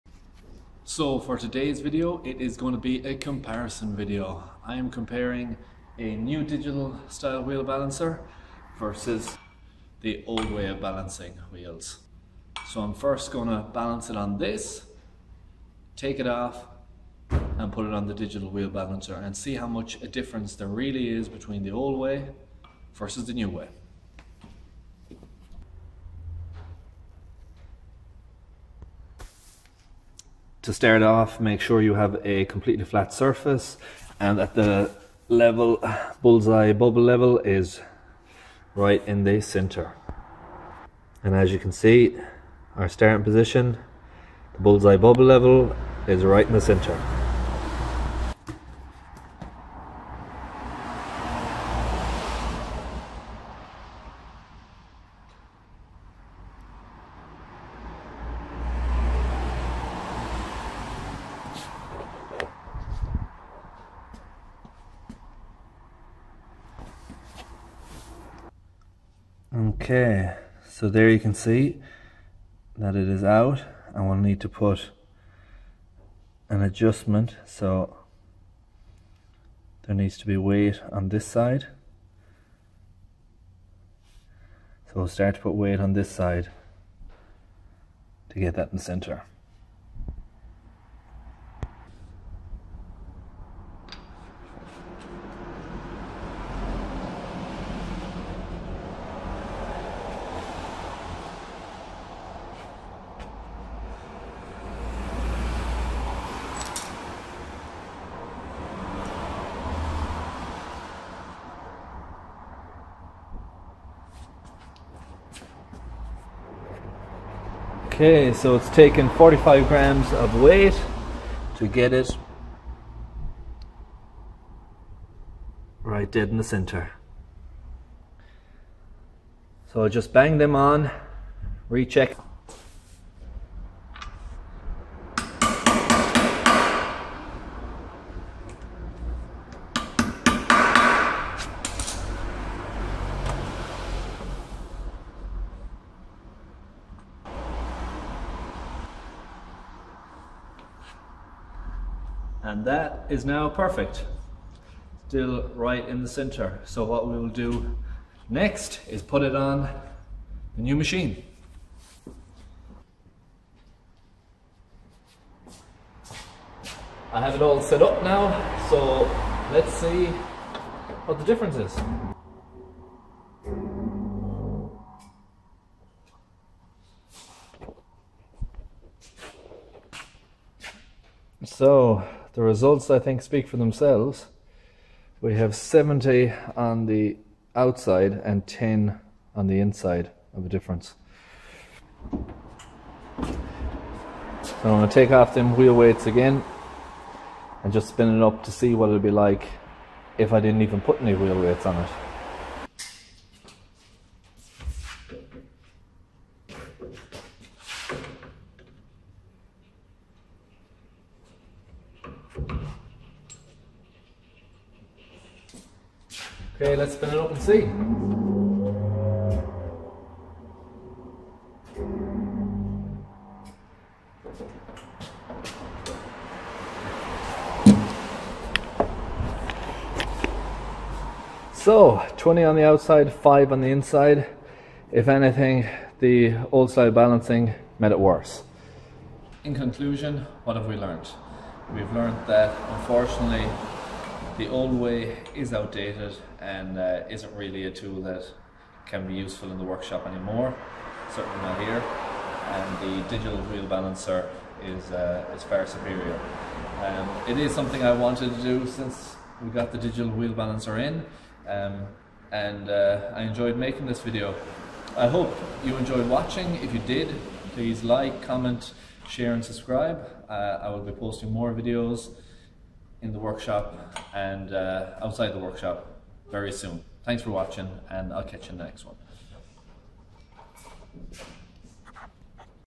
Digital Wheel Balancer Vs Bubble Balancer - Is there much of a difference???
A comparison video showing the old method of wheel balancing V new method of digital wheel balancing. Some old ways are just as good as the new and in this video I balance up the rim using the old method and put it directly on the digital wheel balancer to check if it is out of balance.

Demo wheel balance was done one after the other (old then new) with the portable bullseye balancer perfectly centre before fitting rim and then after applying weights.

I hope you like this video, I enjoyed making it.